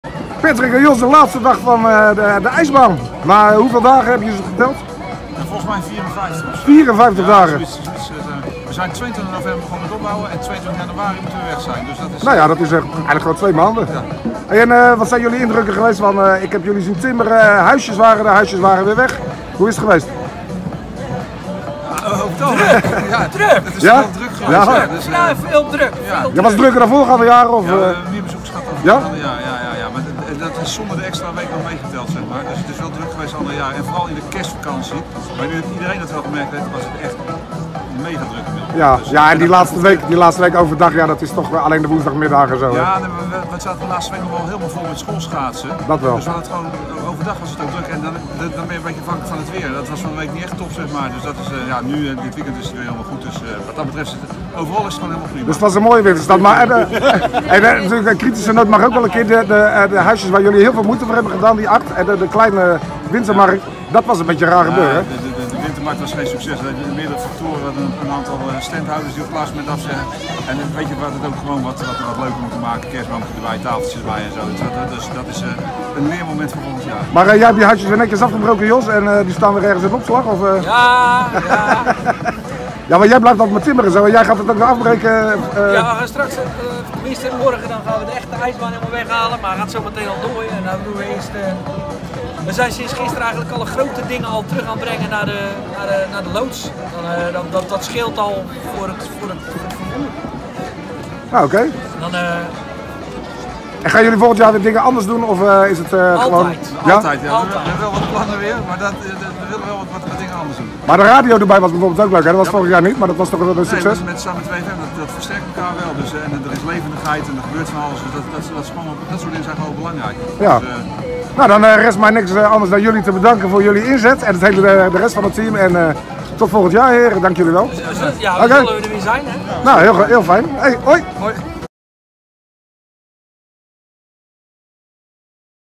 Exit Middelburg Winterstad: Patrick en Jos over 54 dagen IJsbaan Middelburg 2019-2020
Middelburg Winterstad zit erop. Vandaag wordt de ijsbaan afgebroken. Aanstaande donderdag staat Weekmarkt Middelburg weer in de vertrouwde opstelling op de Markt.

Patrick en Jos van Zeeland houdt van schaatsen blikten gisteren kort terug op die 54 dagen dat de ijsbaan op het plein stond. Ze zijn tevreden, maar zien uiteraard ook enkele verbeterpuntjes.

We danken het hele team - en alle sponsoren en andere betrokkenen - hartelijk voor de inzet. Fijn dat de ijsbaan er weer was!